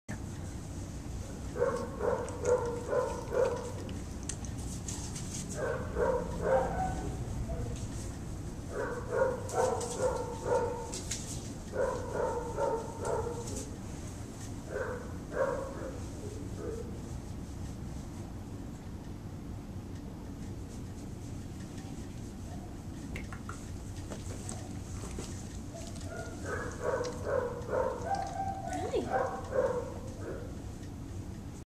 Meet Betsy a Terrier, Pit Bull currently available for adoption at Petango.com! 1/5/2011 8:01:51 PM
Hi, I'm Betsy. I am a shy 6 month old puppy that would rather be in your lap than running around acting like a crazy puppy.  I am timid and shy and I little clingly.  You go to the next room, I go to the next room. I am looking for an owner that will not baby me but will provide me with boundaries and training classes in order to increase my confidence. Will you be the one?